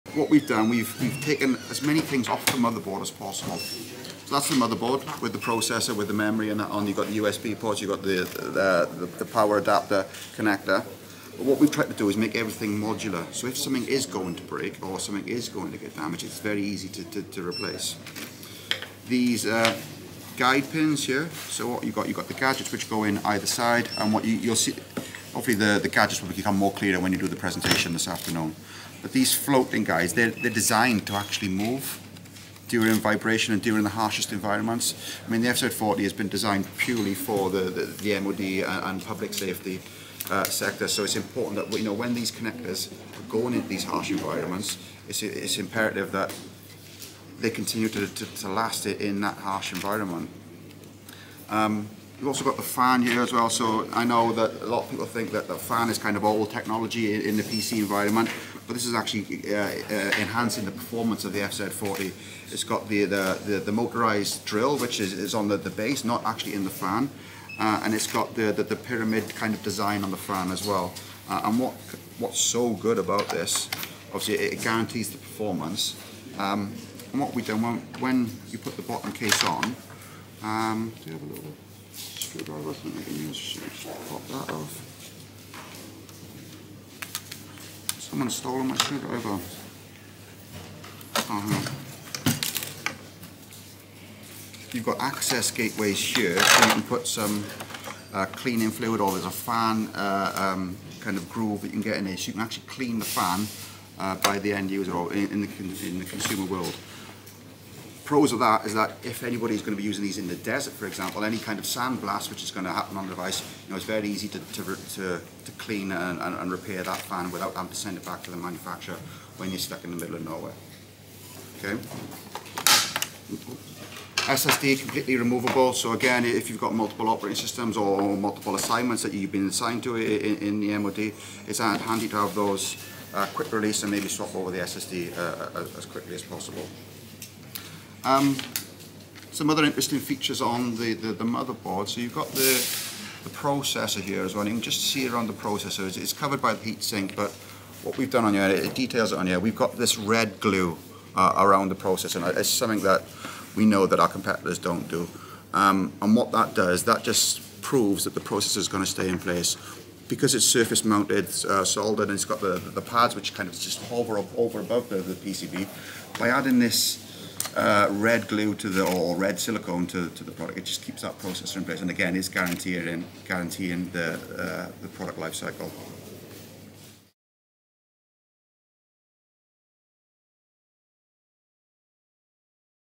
Panasonic Toughbook 40 teardown
The Panasonic Toughbook 40 has been designed to be not only tough but also easy to repair if something goes wrong. Here, the service manager of Panasonic's European operation goes through a torn down machine, component by component.